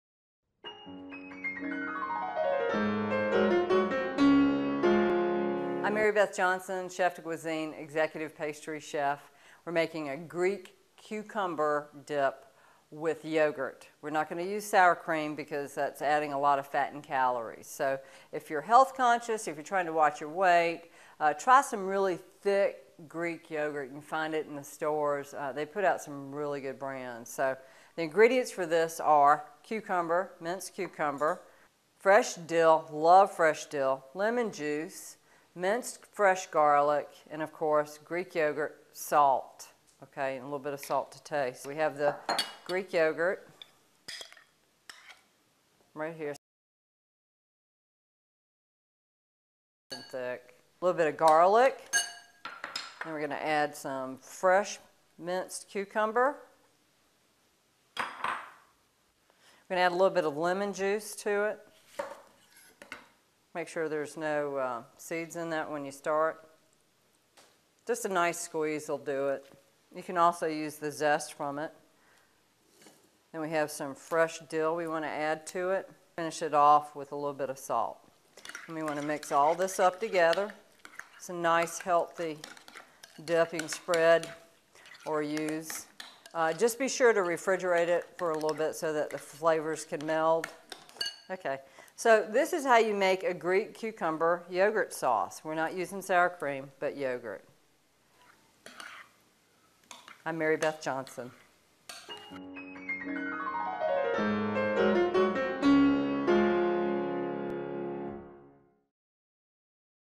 Greek Cucumber & Yogurt Dip Recipe Without Sour Cream
Greek Cucumber & Yogurt Dip Recipe Without Sour Cream. Part of the series: Greek Dips & Pita Meals. Not all Greek cucumber and yogurt dip recipes need to use sour cream as a primary ingredient. Make Greek cucumber and yogurt dip from a recipe that doesn't use sour cream with help from a Chef de Cuisine in this free video clip.